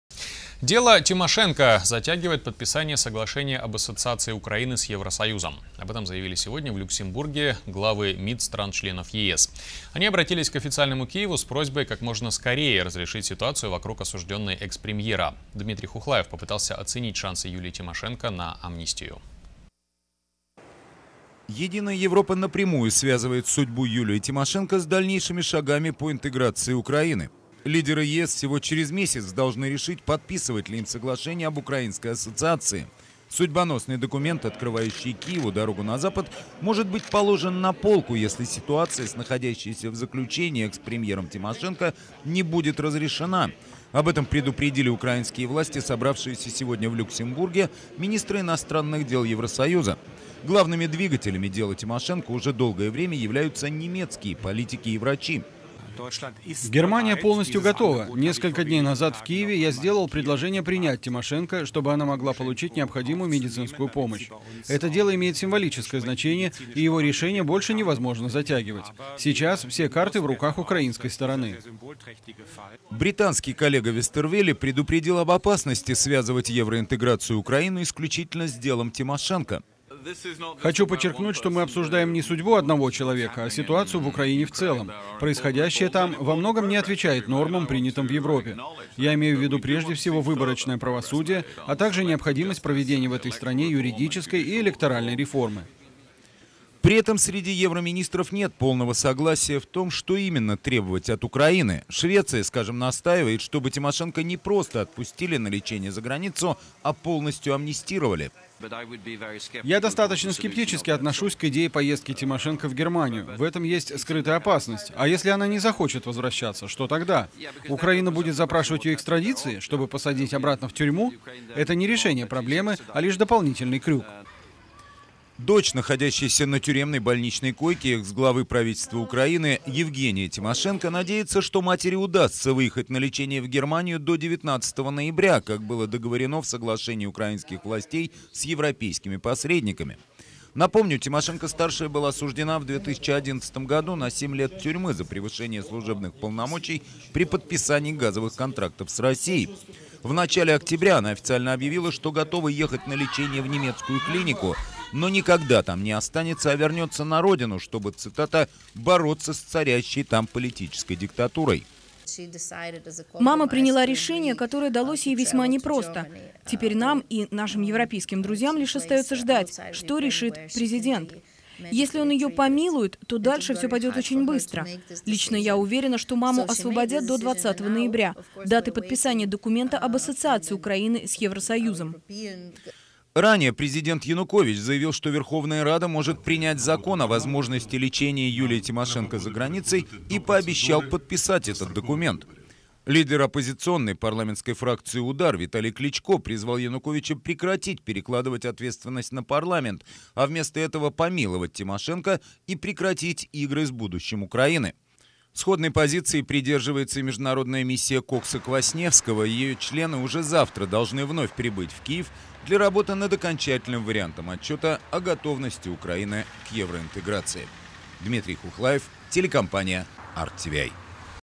Женщина с косой на пути евроинтеграции Украины
"Дело Тимошенко" затягивает подписание соглашения об ассоциации Украины с Евросоюзом. Об этом заявили сегодня в Люксембурге главы МИД стран-членов ЕС. Они обратились к официальному Киеву с просьбой как можно скорее разрешить ситуацию вокруг осужденной экс-премьера.